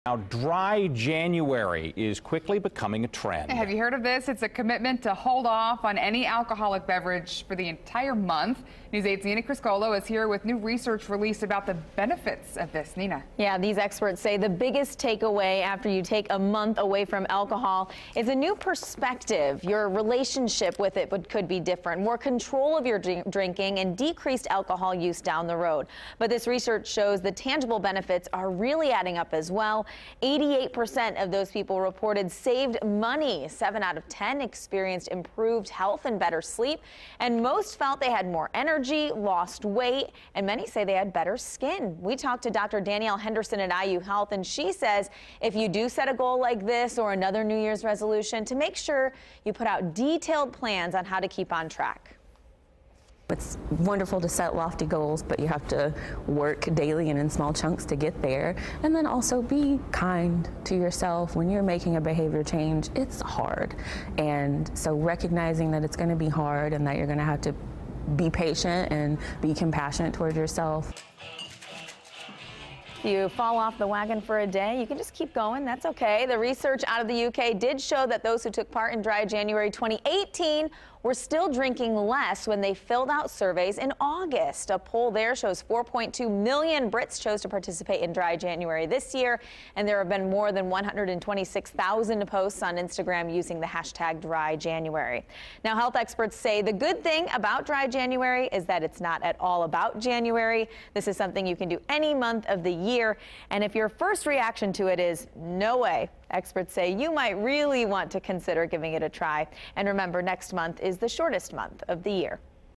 Dry January: Could you do it?
Recently released research results show going a month without alcohol can help you lose weight, increase energy, and save money.